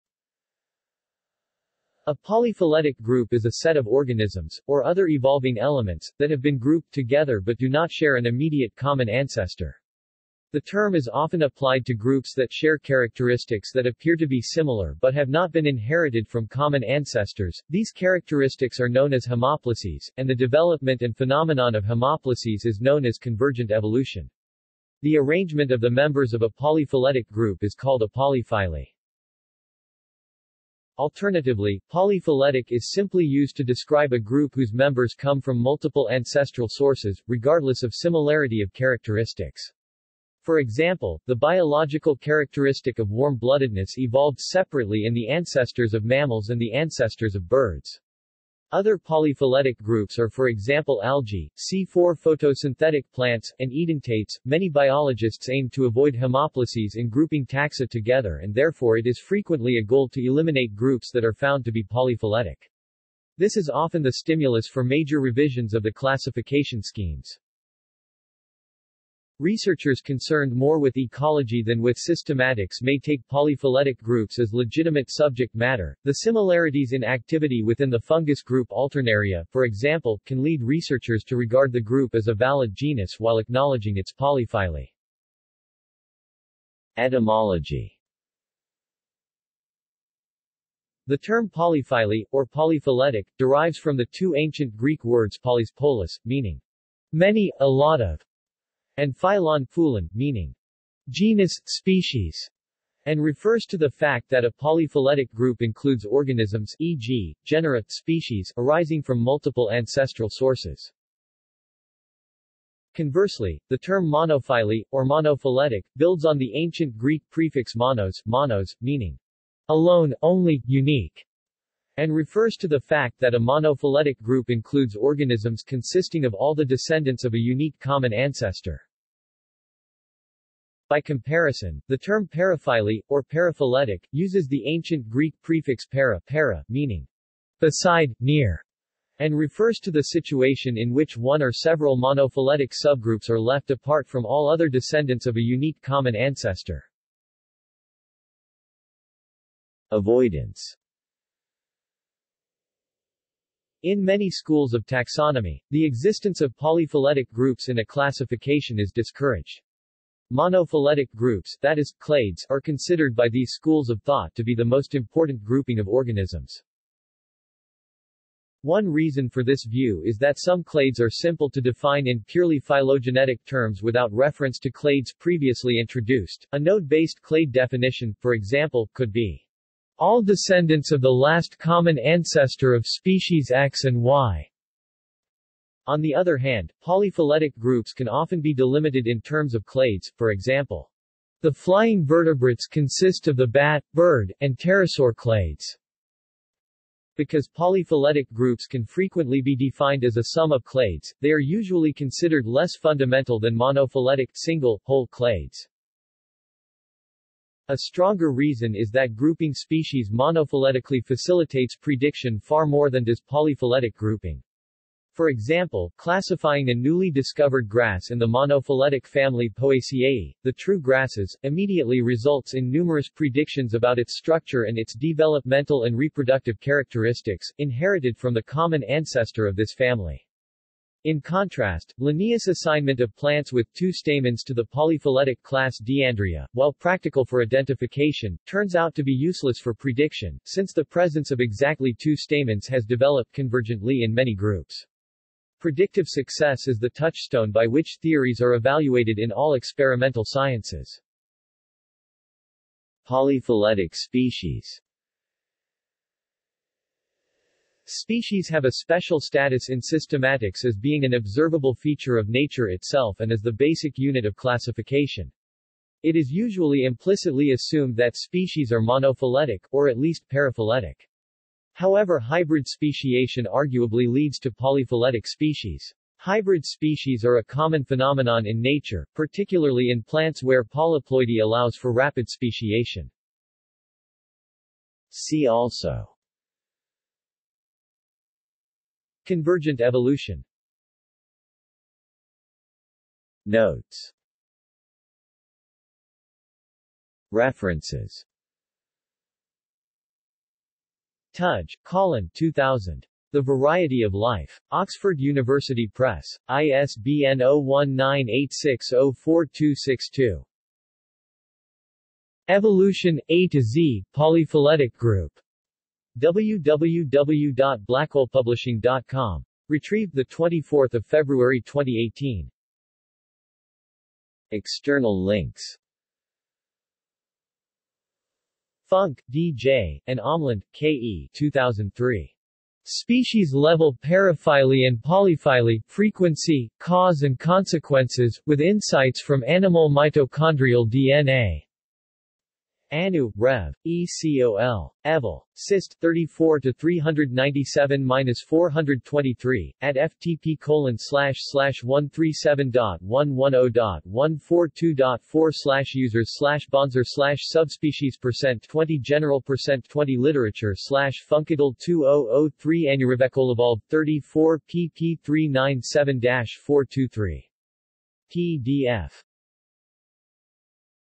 Polyphyletic | Wikipedia audio article
This is an audio version of the Wikipedia Article:
Polyphyletic

SUMMARY
A polyphyletic group is a set of organisms, or other evolving elements, that have been grouped together but do not share an immediate common ancestor. The term is often applied to groups that share characteristics that appear to be similar but have not been inherited from common ancestors; these characteristics are known as homoplasies, and the development and phenomenon of homoplasies is known as convergent evolution. The arrangement of the members of a polyphyletic group is called a polyphyly.
Alternatively, polyphyletic is simply used to describe a group whose members come from multiple ancestral sources, regardless of similarity of characteristics. For example, the biological characteristic of warm-bloodedness evolved separately in the ancestors of mammals and the ancestors of birds. Other polyphyletic groups are for example algae, C4 photosynthetic plants, and edentates.Many biologists aim to avoid homoplasies in grouping taxa together and therefore it is frequently a goal to eliminate groups that are found to be polyphyletic. This is often the stimulus for major revisions of the classification schemes.
Researchers concerned more with ecology than with systematics may take polyphyletic groups as legitimate subject matter; the similarities in activity within the fungus group Alternaria, for example, can lead researchers to regard the group as a valid genus while acknowledging its polyphyly.